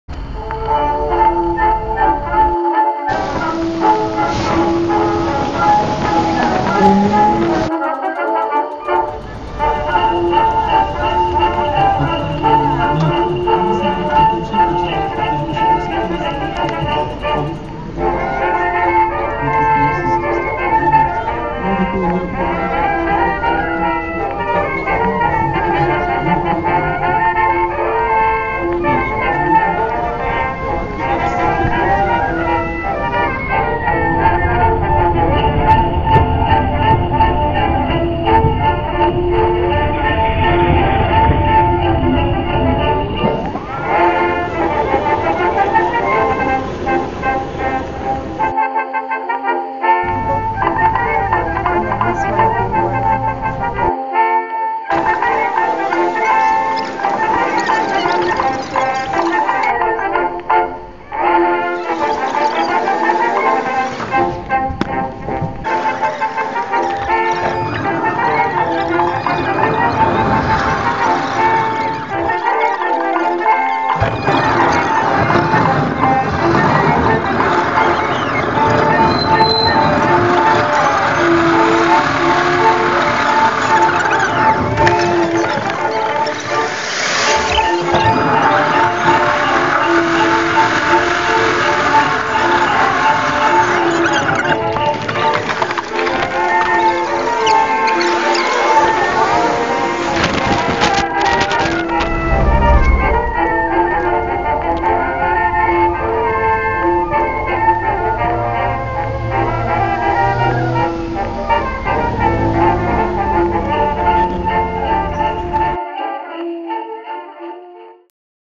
Walking in Vinohrady
Testing my new Polaroid CUBE and trying to learn how to edit videos.